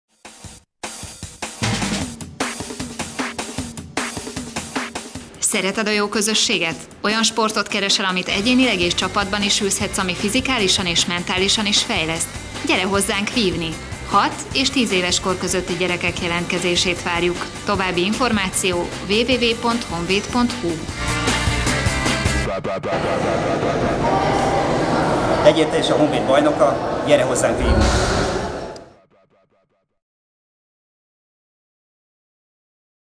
Gyere vívni a Honvédba! 🤺
Kárpáti Rudolf, Dr. Nébald György … 🥇Nagy Tímea … Dr. Nébald Rudolf, Szelei István, ❤️ Jánosi Zsuzsanna, 👑Imre Géza, Boczkó Gábor, ha ismered ezeket a neveket, akkor tudod, hogy mind neves, sikeres vívó világklasszisok. Vívó Szakosztályunk megalakulása óta töretlenül a legjobbak közt vannak az olimpiákon és világversenyeken.

Ez a több száz éves hagyományokkal rendelkező sport korábban a nemesek kiváltsága volt, és a mai versenyzők mozgása rendkívül kifinomult és látványos.

Asszó, csel, kavacio, parád, riposzt és megannyi szakkifejezést 📔meg fogsz tanulni és egyéniben vagy csapatban is versenyezhetsz, ha kedved van.

Ha lejönnél megnézni egy edzést, akkor egyesületünkben kézbe is foghatod a fegyvereket 🗡és megláthatod, hogy jó társaságban zajlanak a délutáni edzések.

Gyere, próbáld ki!

Jelentkezés a alatt található elérhetőségeinken 📲 📩 lehet.